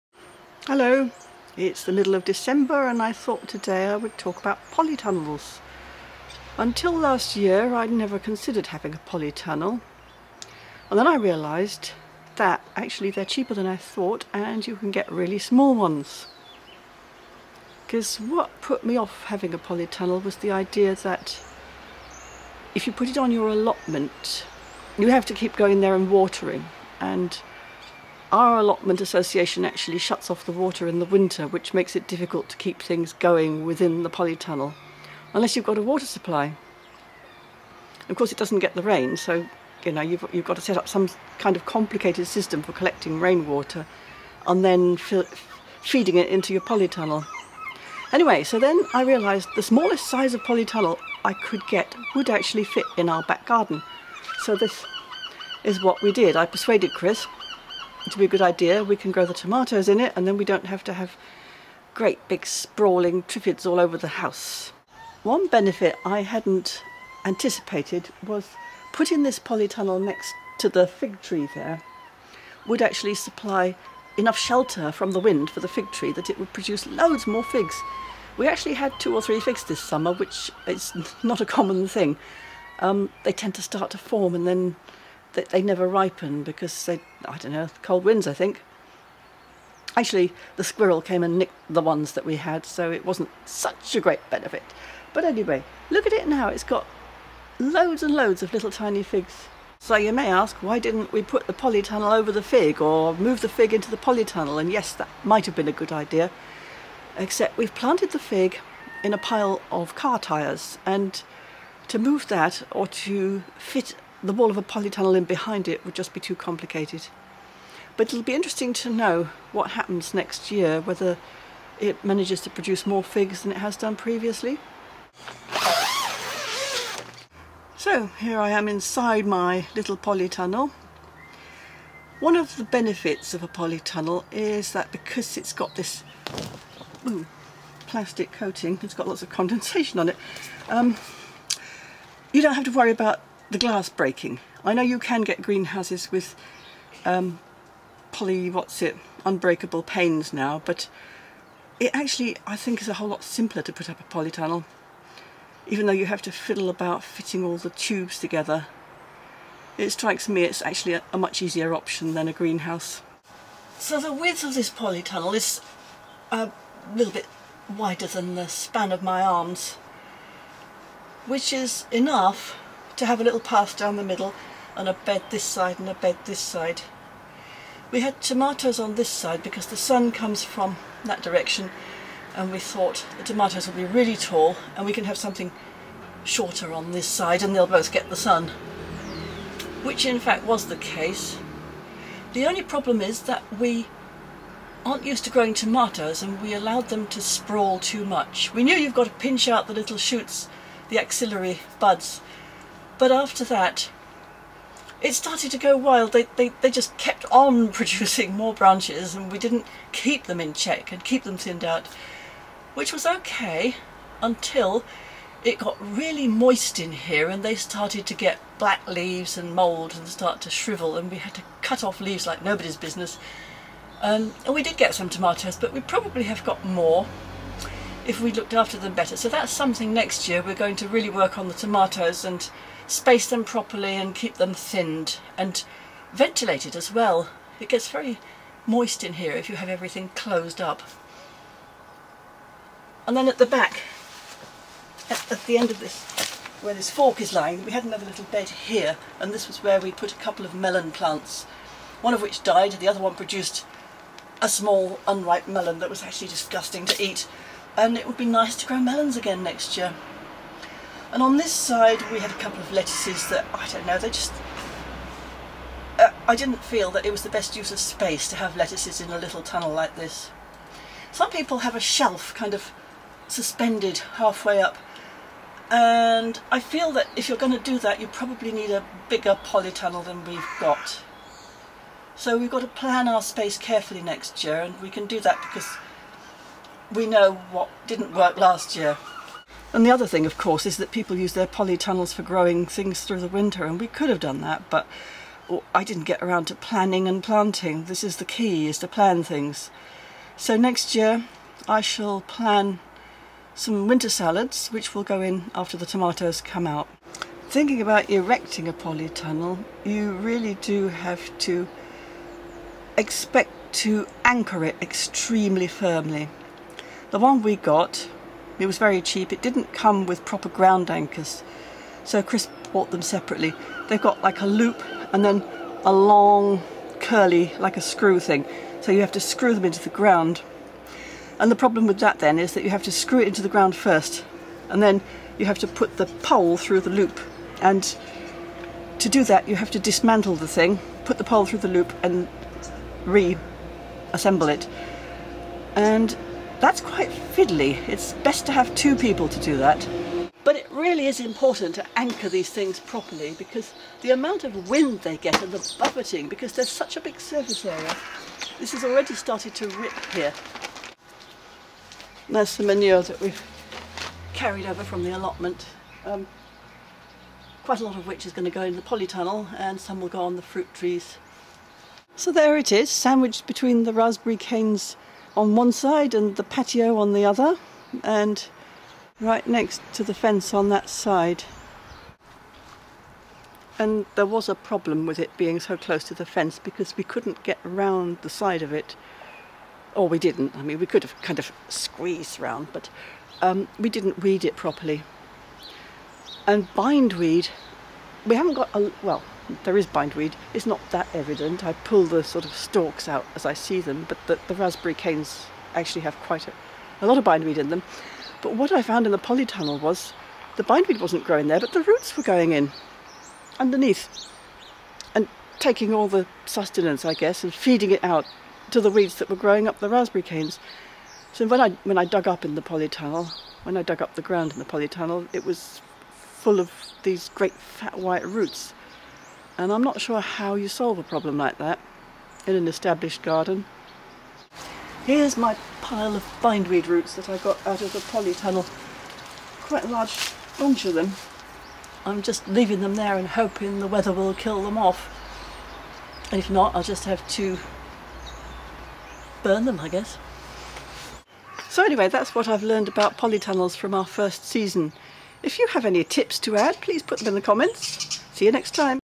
Getting a polytunnel
Some considerations and our experience of the first year with a polytunnel in the back garden!